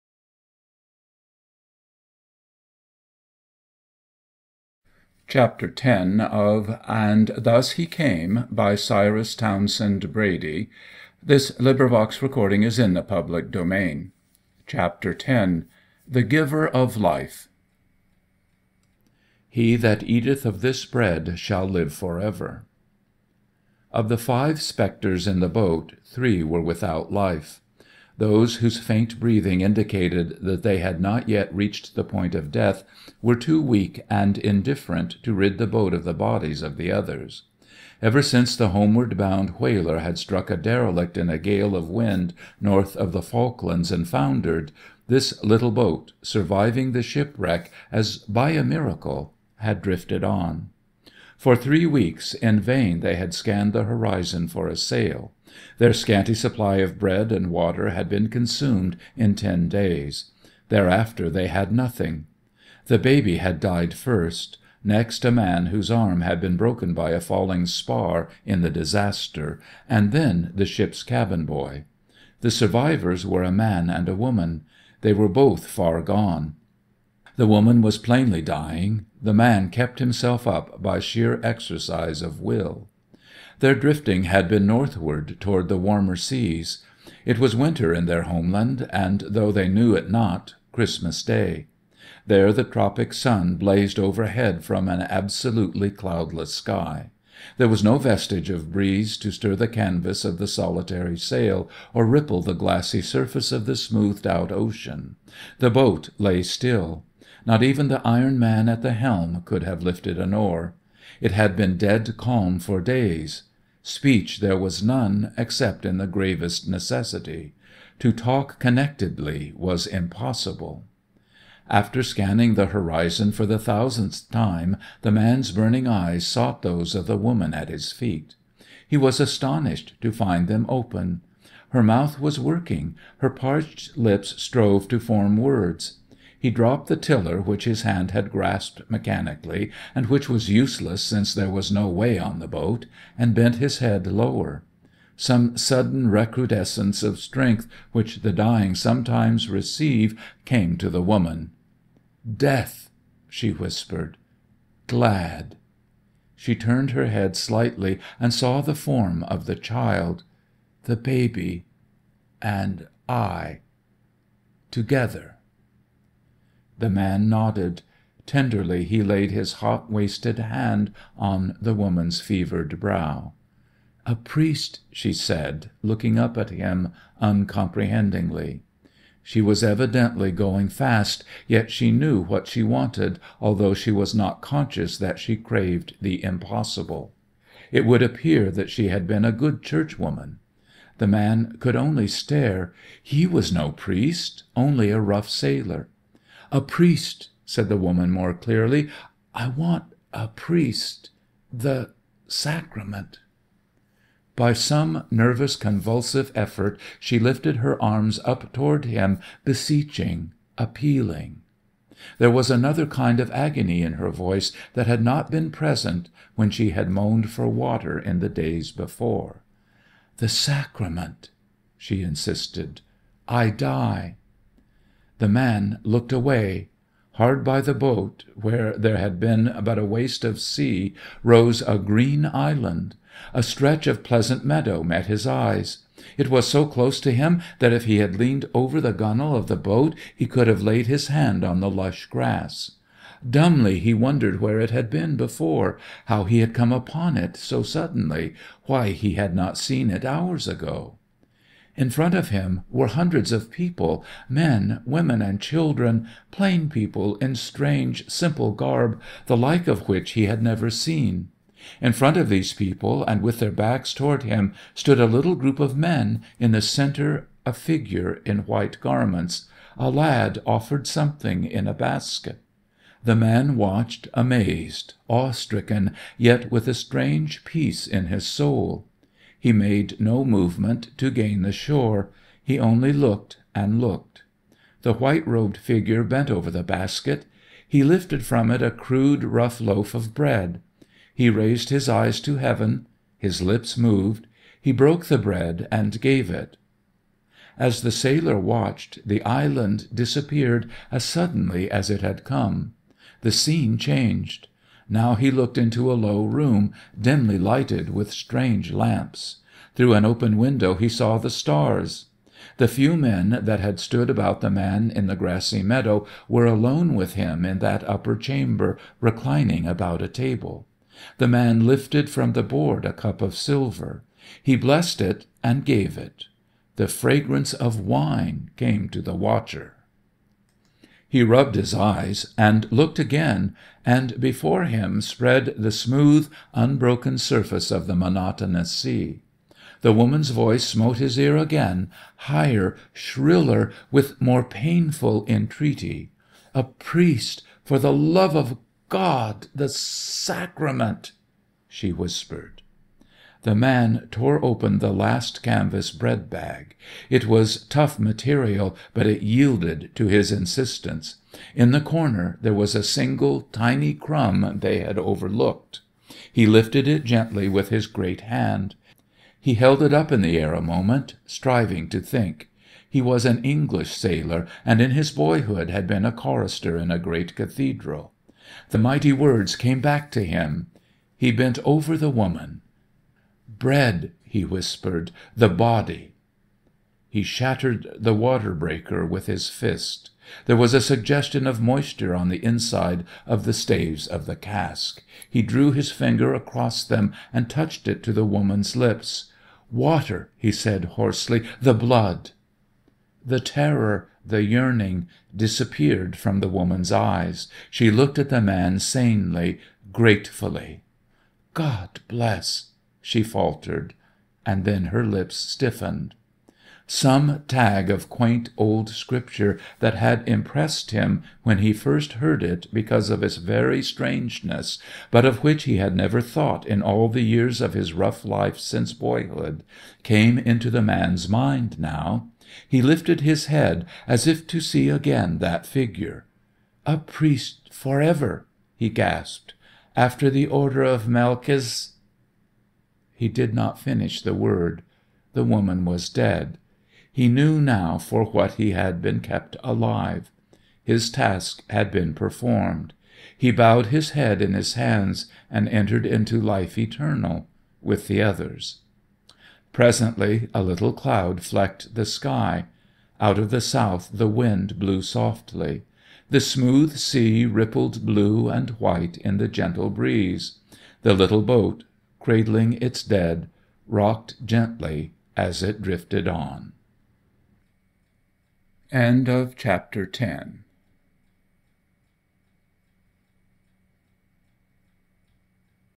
And Thus He Came by Cyrus Townsend Brady - 10/11. The Giver of Life (read by David Wales)
LibriVox recording of And Thus He Came by Cyrus Townsend Brady. Read in English by David Wales.

Cover design by Kathryn Delaney, illustration from the first edition. This design is in the public domain.